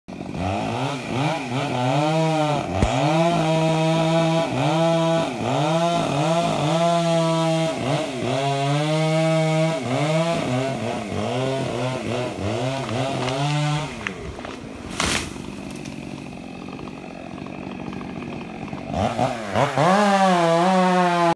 Brimfield Massachusetts Tree Service - Running Brook Tree Climbers
The Running Brook Team taking down trees in Brimfield Massachusetts. Perfect landing! If you need quality Tree Removal in Brimfield, Give us a shout!